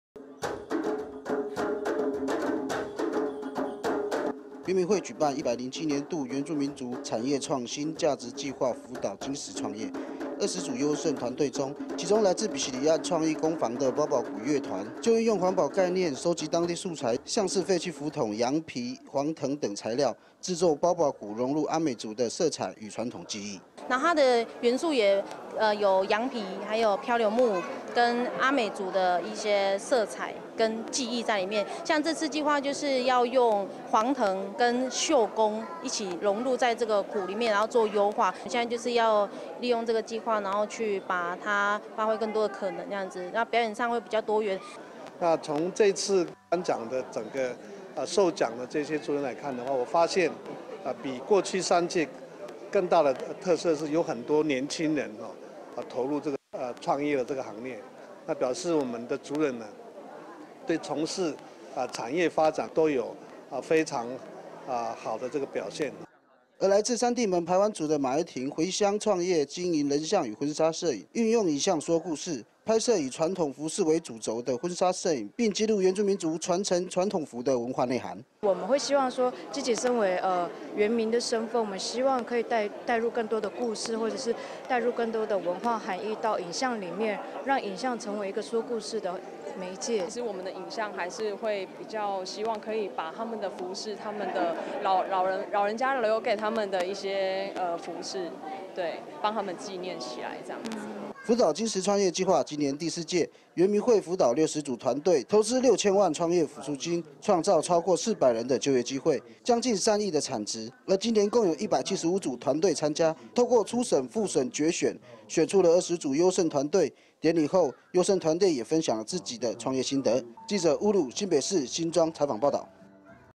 產業創新創業計畫頒獎 優勝團隊談夢想 2018-10-18 IPCF-TITV 原文會 原視新聞
原民會舉辦「107年度原住民族-產業創新-價值計畫-輔導精實創業」，20組優勝團隊中，其中來自比西里岸創意工坊的PawPaw鼓樂團，就運用環保概念，收集當地素材，像是廢棄浮筒、羊皮、黃藤等材料，製作PawPaw鼓融入阿美族的色彩與傳統技藝。

而來自三地門排灣族的馬郁婷，回鄉創業經營人像跟婚妙攝影，運用影像說故事，拍攝以傳統服飾為主軸的婚紗攝影，並記錄原住民族傳承傳統服飾的文化內涵。

「輔導精實創業計畫」今年第四屆，原民會輔導60組團隊，投資6,000萬創業補助金，創造超過400人的就業機會，將近3億元的產值，而今年共有175組團隊參加，透過初審、複審及決選，選出了20組優勝團隊，典禮後，優勝團隊也分享自己的創業心得。